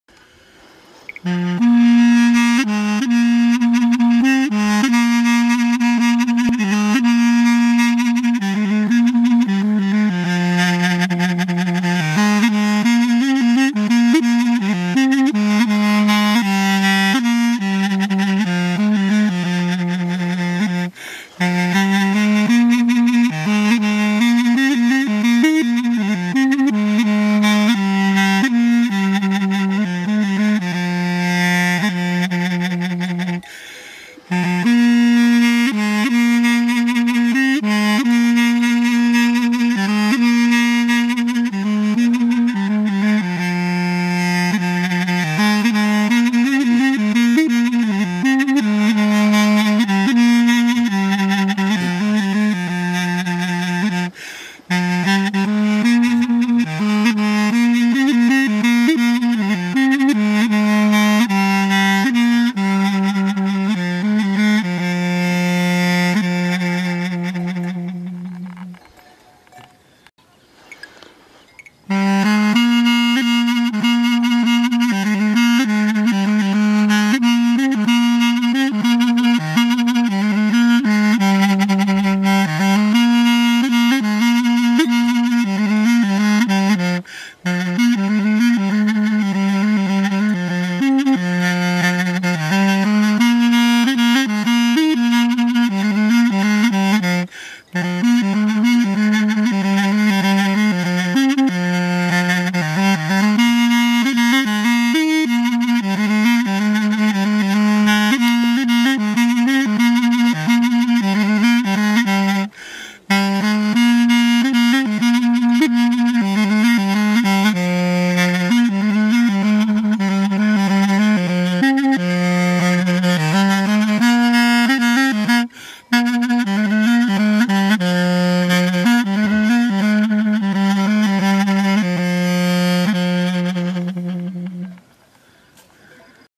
TRT Sanatçsı Zafer Daşdan'dan güzel bir dinleti
TRT Sanatçsı Zafer Taşdan'dan muhteşem manzara eşliğinde  güzel bir dinleti